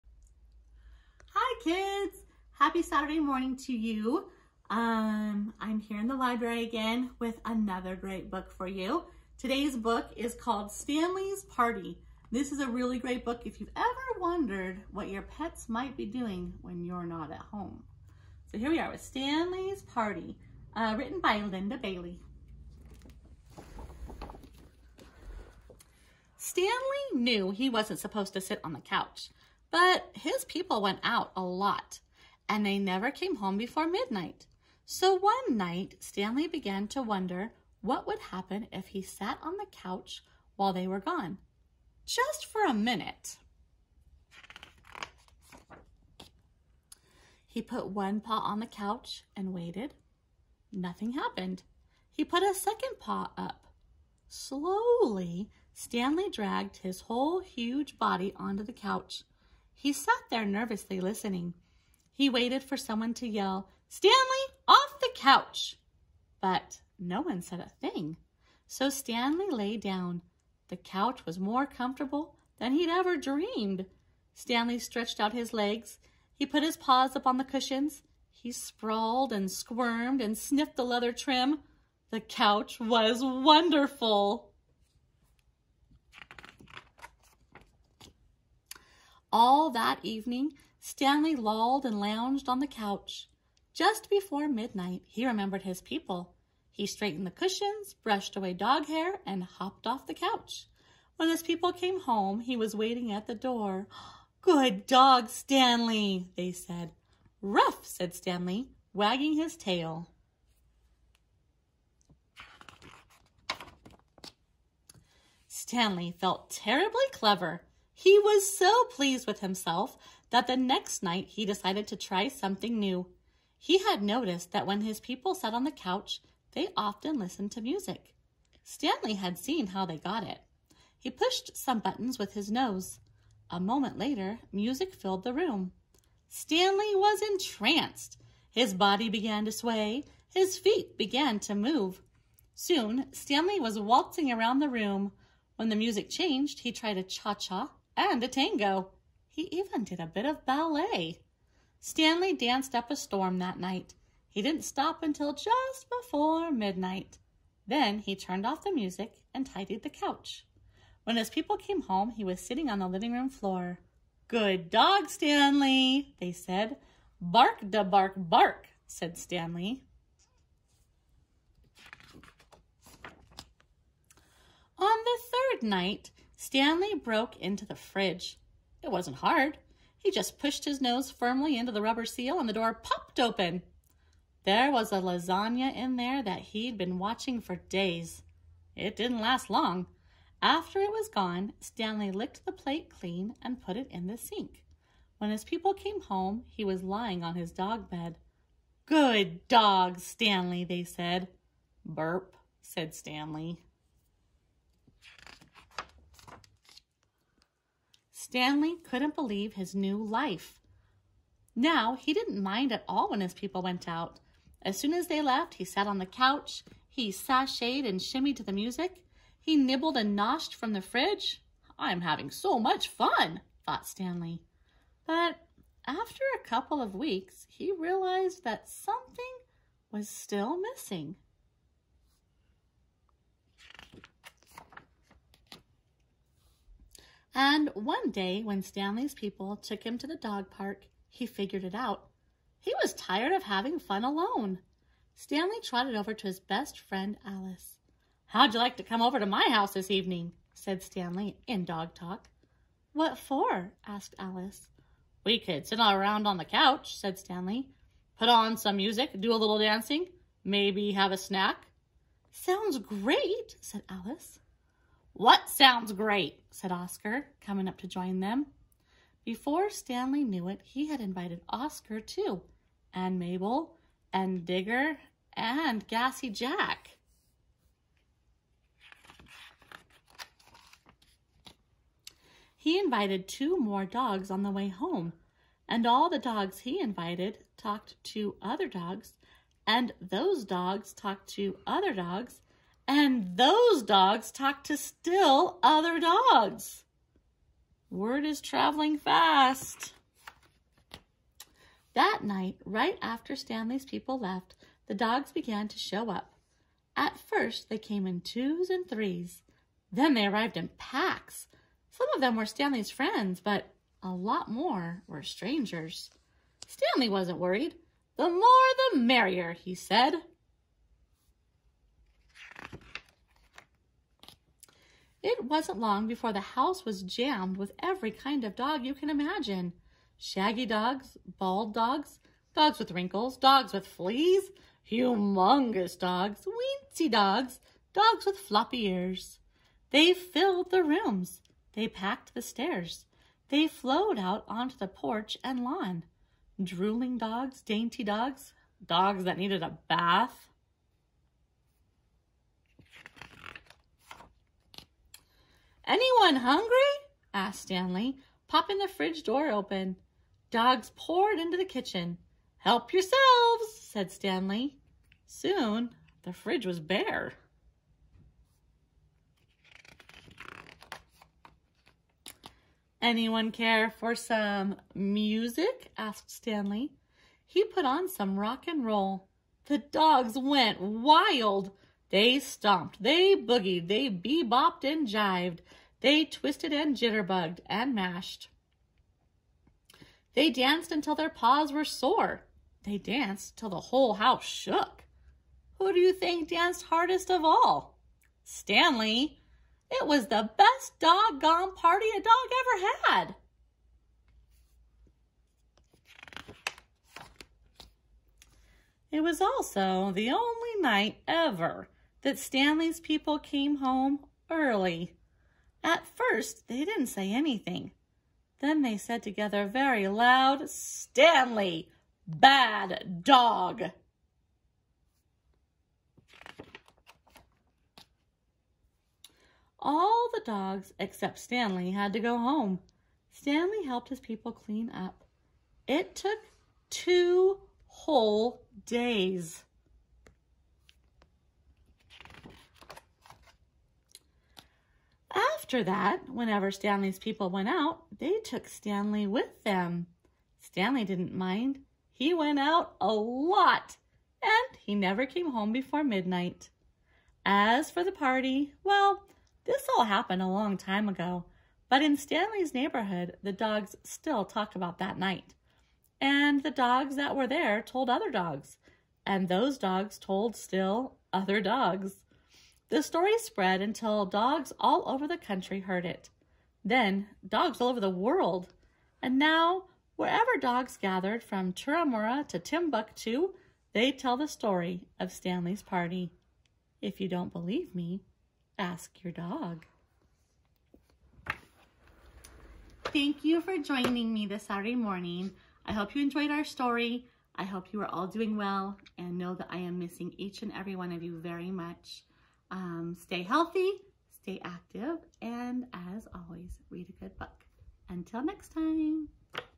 Mrs.Brown’s Saturday Morning Story Time
This morning Mrs. Brown is reading “Stanley’s Party” by Linda Bailey!